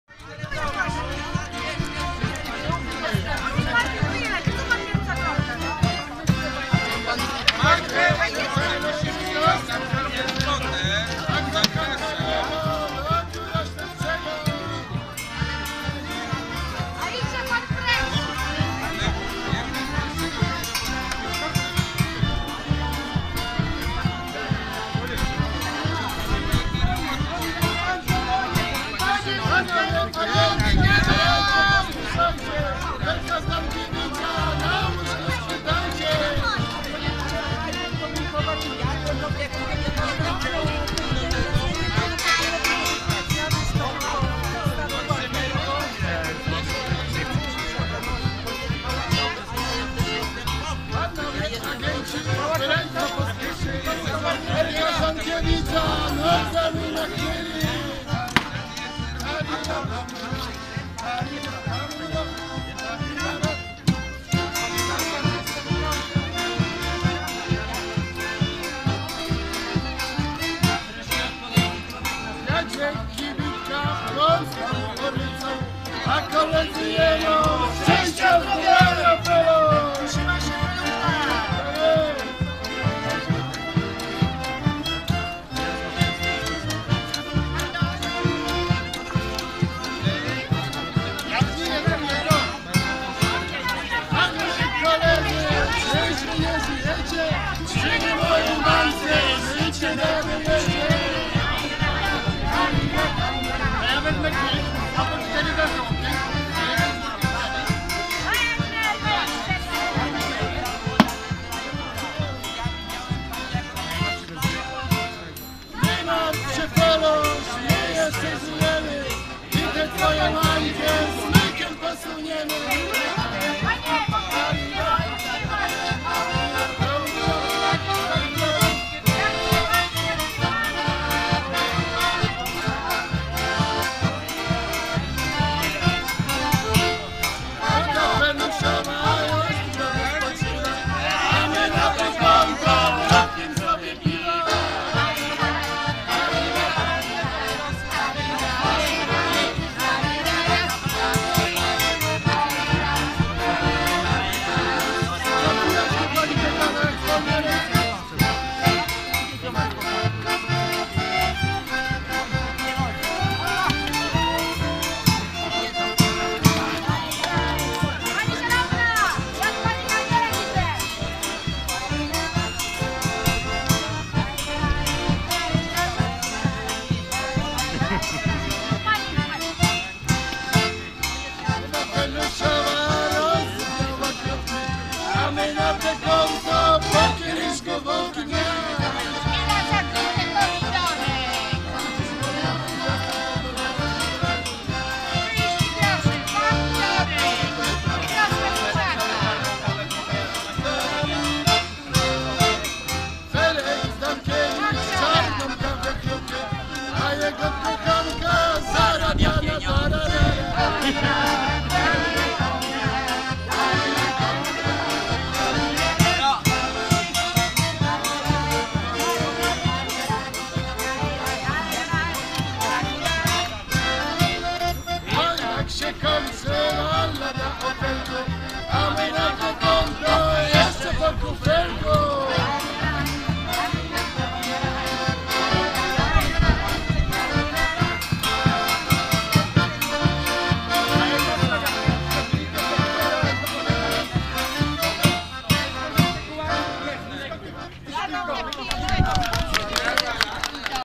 Grupa Teatralna Warszawiaki. Kercelak 2016 r.
W święto Woli organizowane przez Urząd Dzielnicy Wola m.st. Warszawy pierwsze skrzypce grała Grupa Teatralna Warszawiaki.
Przygotowali kawałek przedwojennej Warszawy w klimacie bazaru Kercelego, który był największym bazarem w Warszawie.
Znajdował się w okolicy ulic Leszno, Okopowa, Wolska i Chłodna.
Warszawiaki przywrócili klimat tamtych dni piosenką, skeczami, gwarą warszawską.
Grupa Teatralna Warszawiaki to jeden z najlepszych teatrów ulicznych oddających klimaty Warszawy.